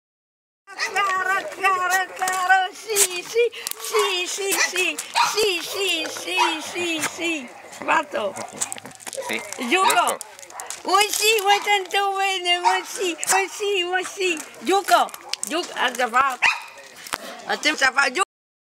Zucchero il cane che ride
Zucchero, il cane di Mantova che ride - Zucchero, the smiling dog from Mantova Italy.